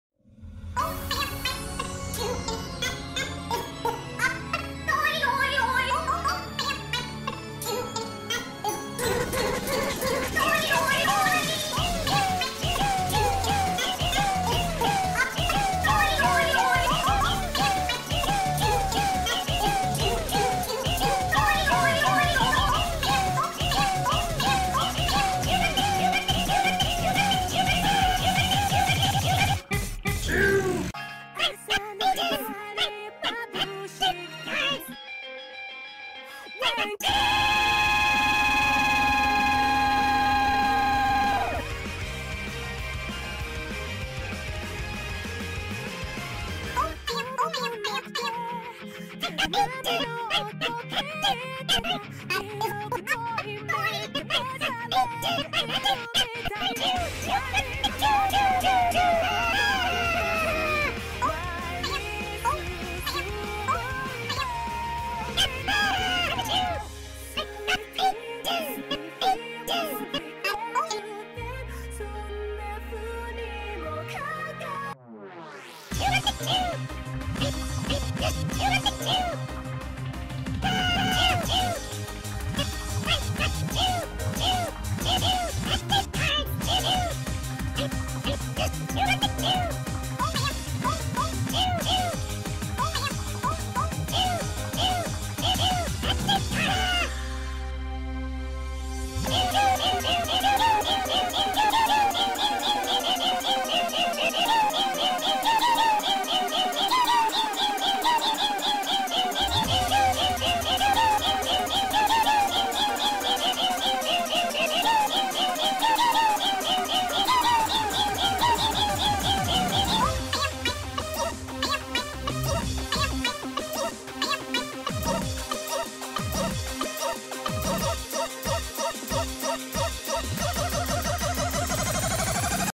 old abandoned shoe remixes
I'm never going to finish any of these but it's probably not good to leave them to rot on my hard drive. Also miiro is terrible

youtube fixed the letterboxing way to go technology